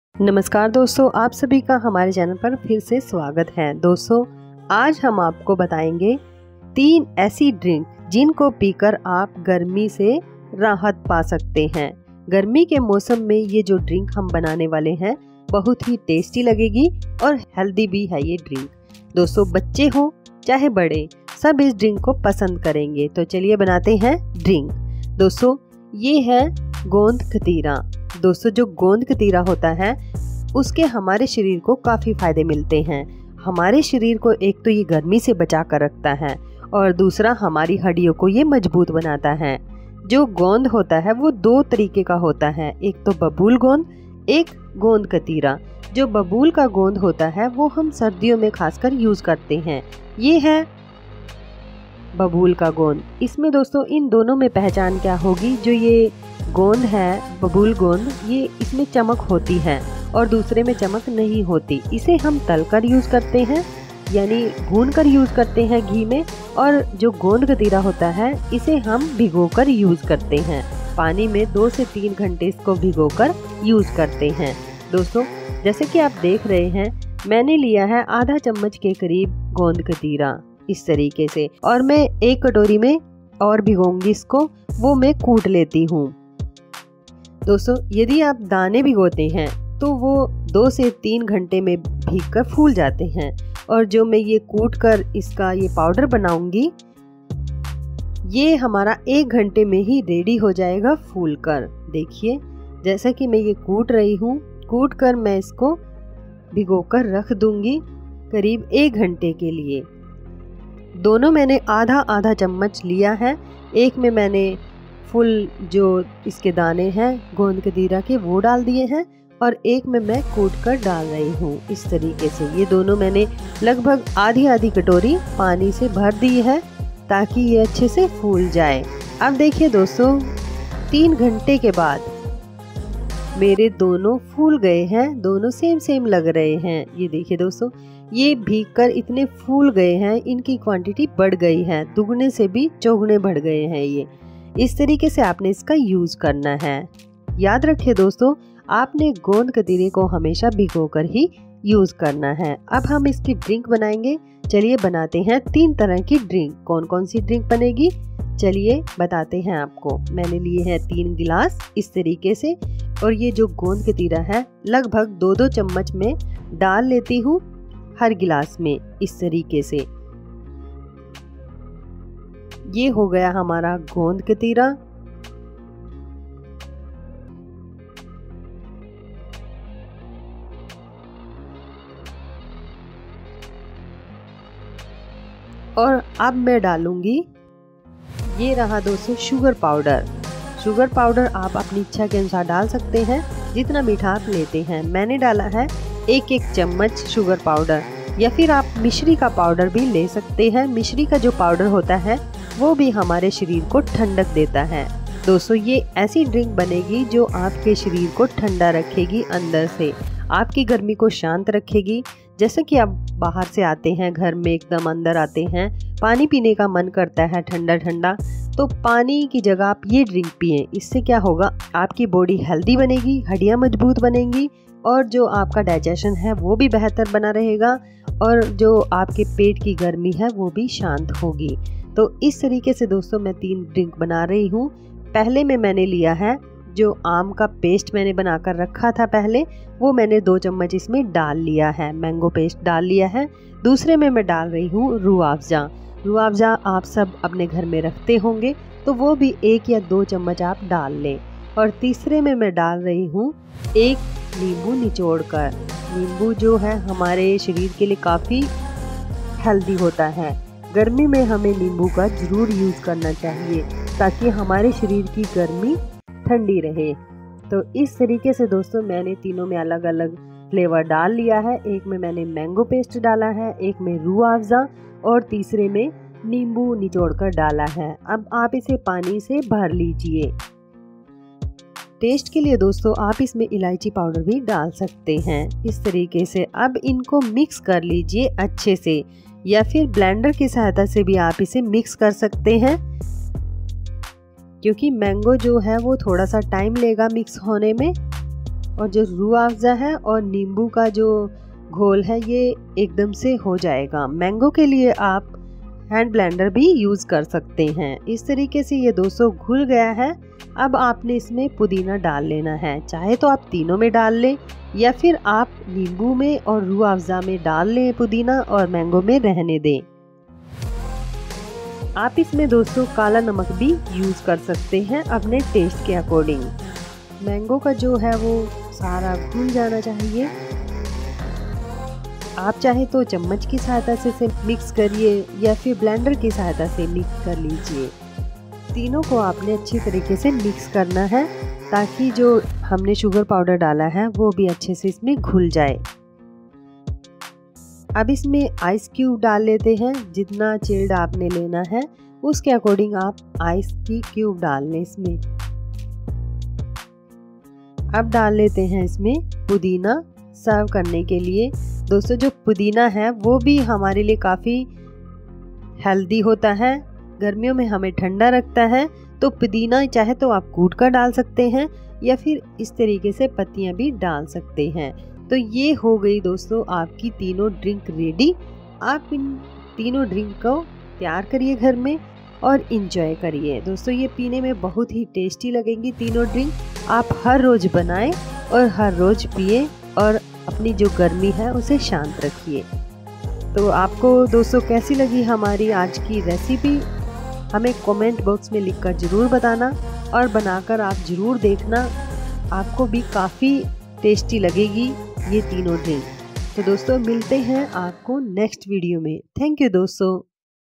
गर्मियों में, कुछ दिन ये पी लो, पेट की गर्मी कोसो दूर भाग जाएगी | 3 Magical Drinks for Summer
आज मैं आपके लिए गोंद कतीरा से बनने वाले flavoured Shakes की रेसिपी लेकर आयी हूँ जो बहुत ही ज़्यादा टेस्टी भी है और साथ ही healthy भी हैं गोंद कतीरा में इतने गुण है कि आप गिन नहीं पाएंगे गर्मियों में ये अमृत के समान है इससे लू नहीं लगती है साथ ही एंटी एजिंग है और बोन्स के लिए भी बहुत अच्छा है| इसे छोटे और बड़े सभी खा सकते हैं |